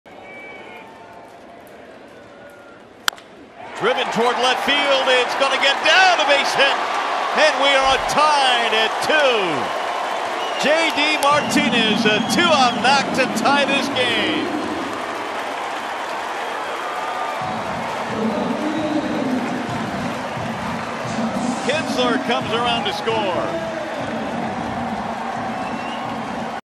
CWS@DET: Martinez ties the game with an RBI single
J.D. Martinez ropes a base hit to left field, scoring Ian Kinsler from third to tie the game at 2 in the 8th